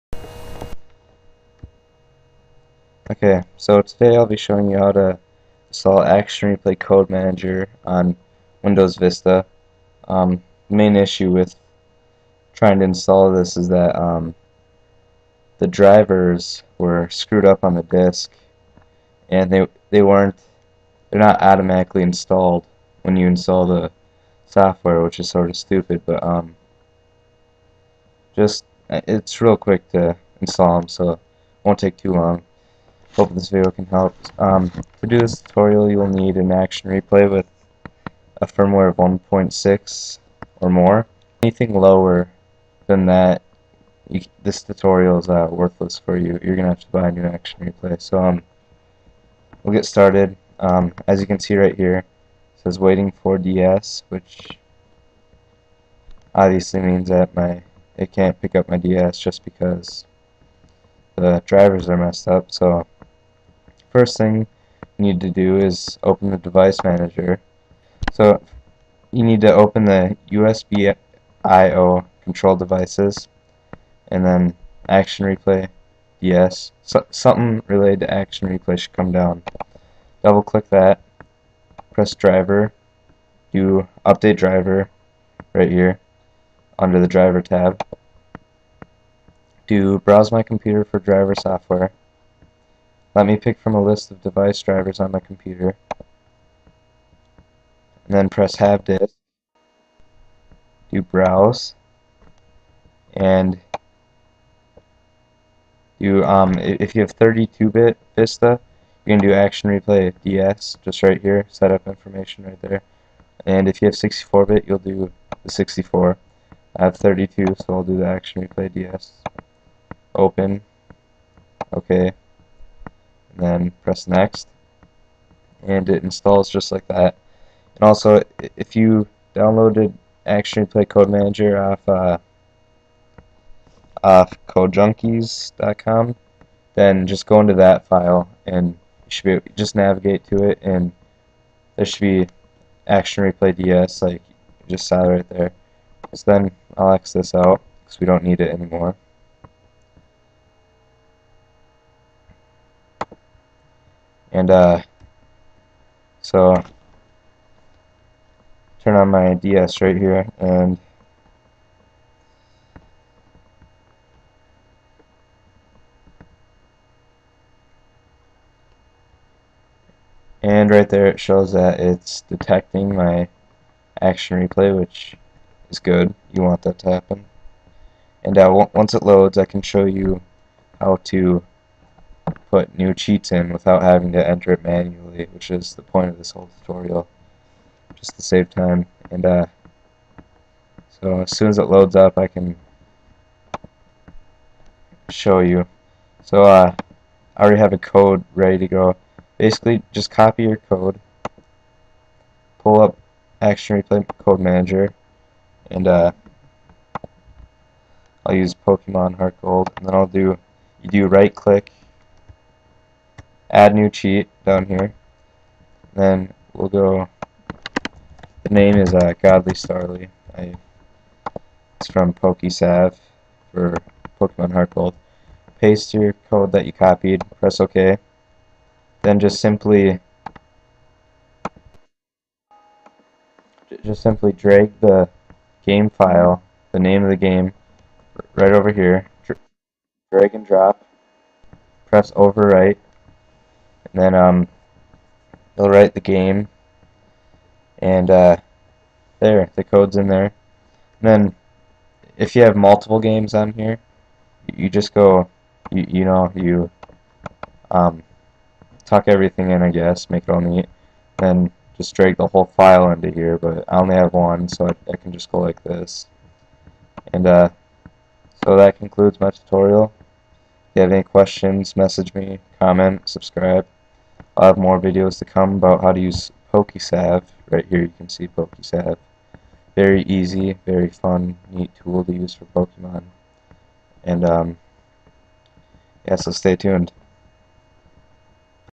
Action Replay Code Manager Driver Updates (Vista)
READ DESCRIPTION!:
SORRY ABOUT THE BAD AUDIO QUALITY :(
In this video i show you how to set up the drivers necessary to run Action Replay Code Manager on your Windows Vista Device.